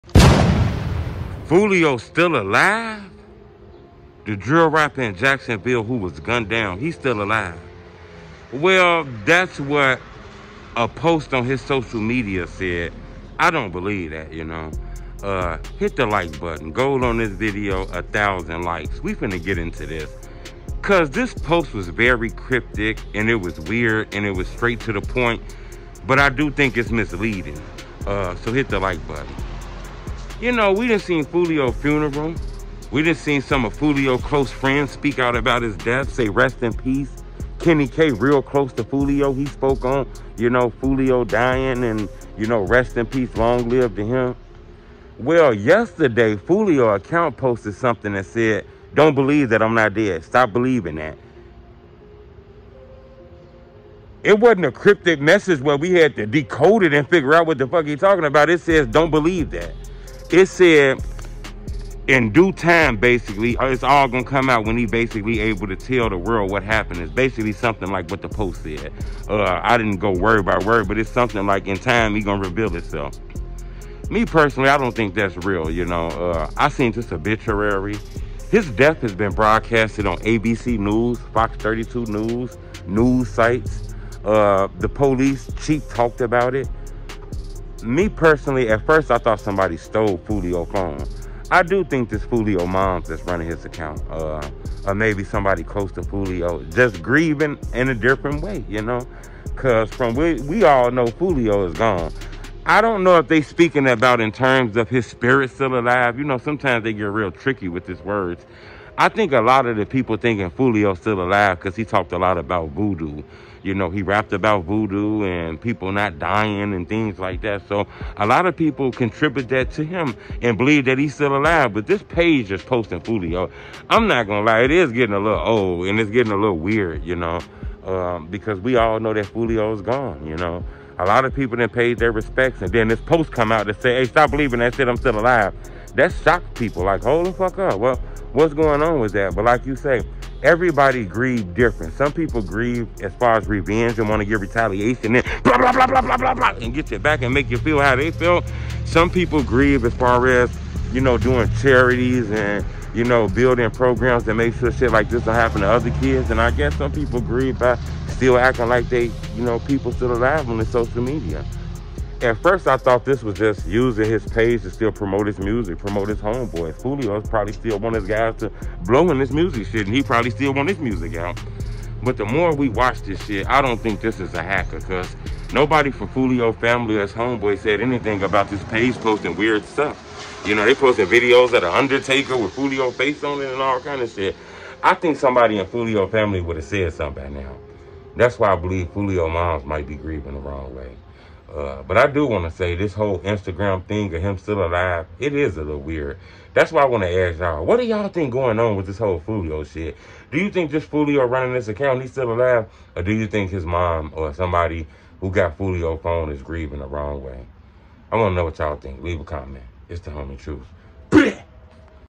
Foolio Faked His Death According To His IG But In Reality He’s Dead, Someones Grieving The Wrong Way
Foolio Faked His According To His IG But In Reality He’s Dead, Someones Grieving The Wrong Way